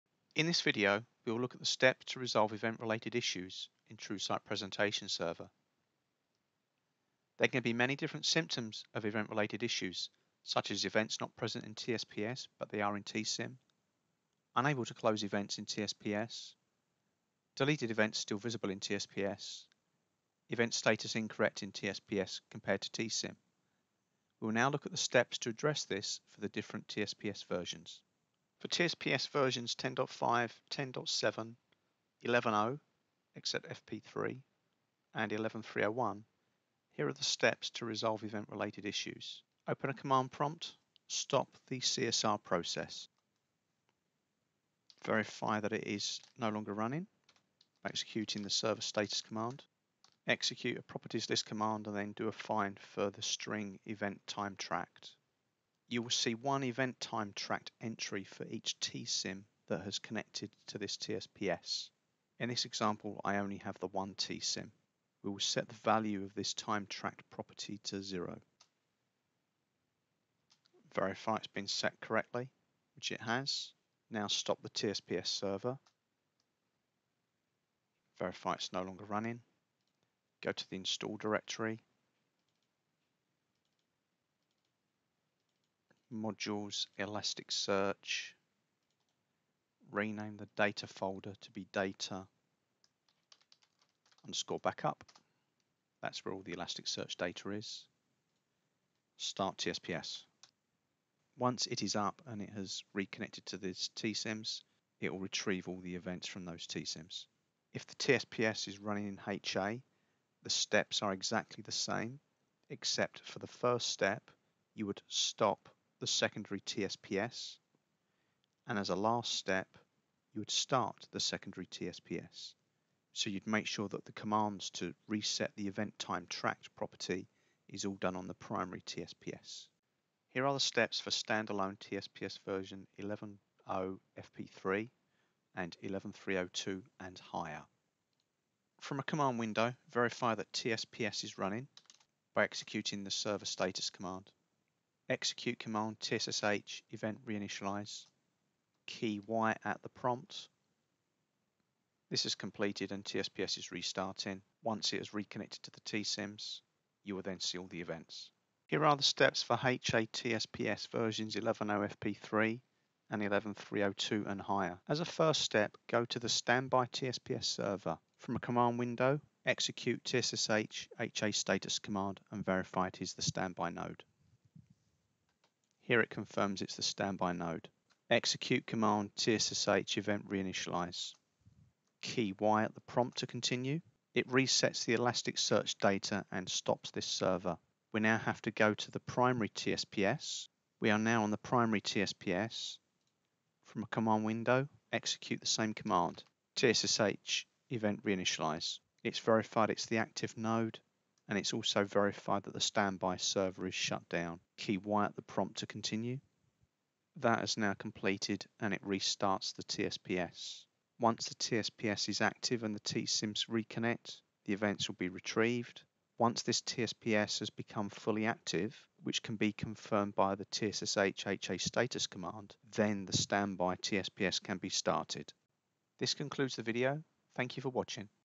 BMC TSOM: How to Resolve Event-Related Issues in TrueSight Presentation Server
Steps for different TSPS versions on how to resolve event sync type issues